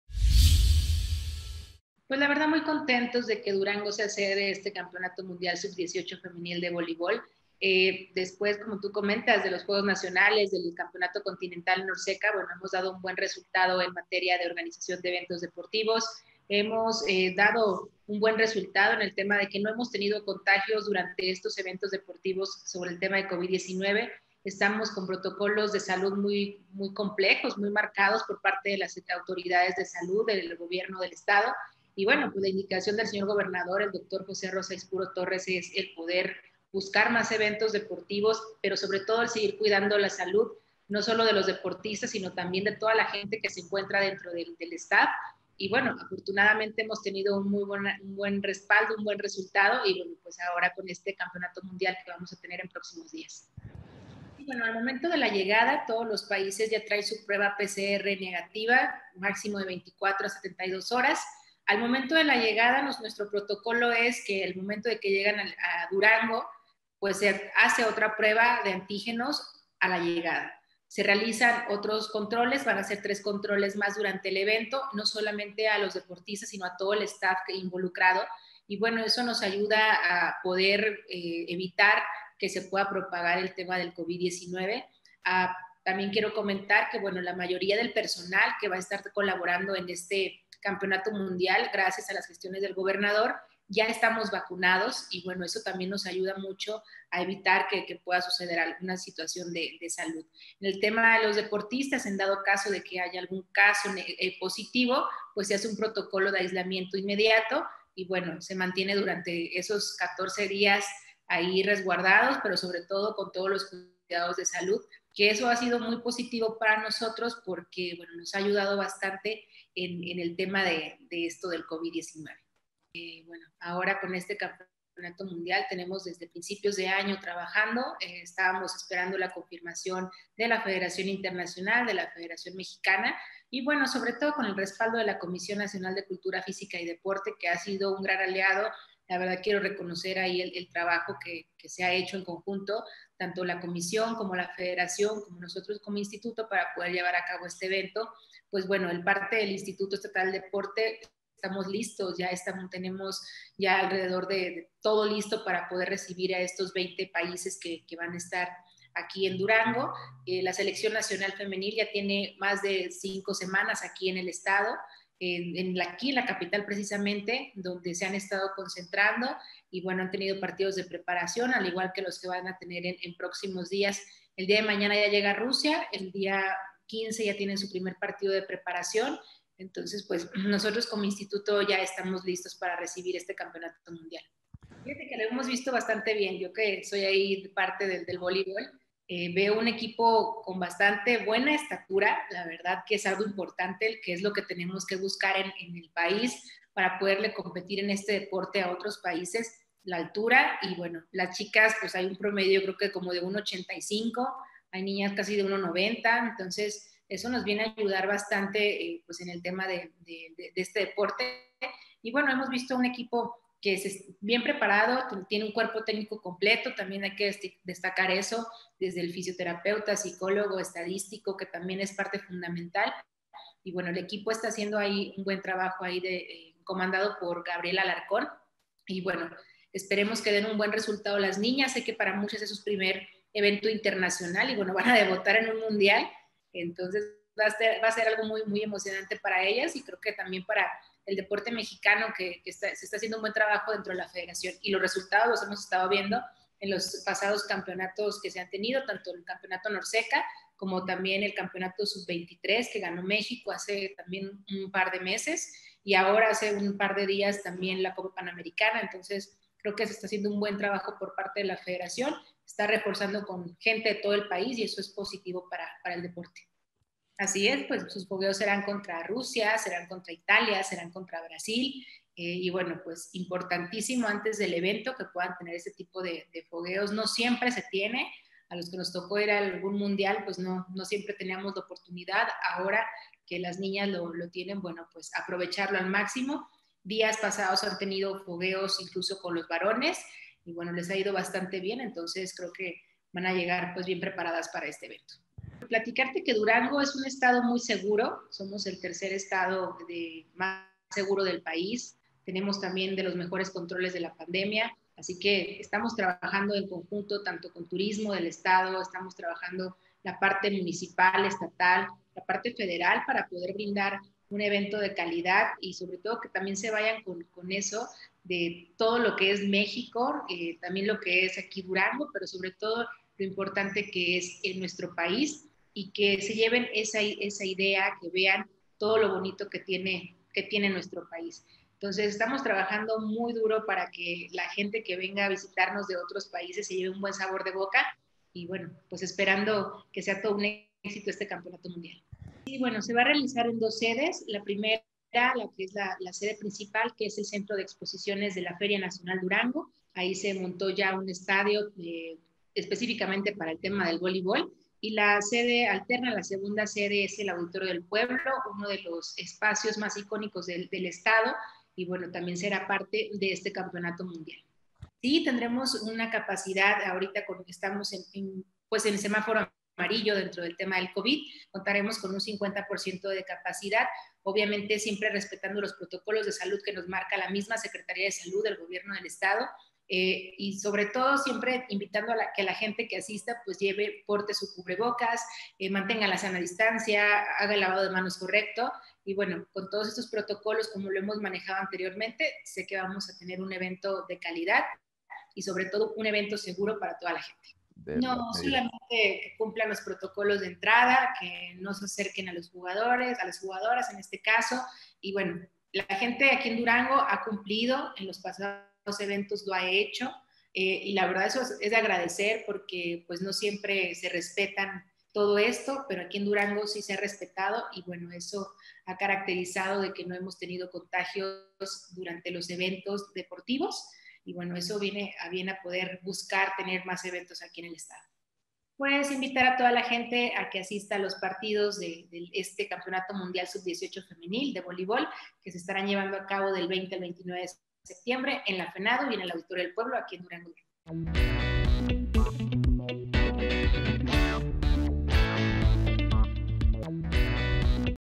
En marcha el Campeonato Mundial de Voleibol Femenino Sub 18 en Durango
La directora del Instituto Estatal del Deporte de dicho estado explicó lo que se vivirá en esta competencia internacional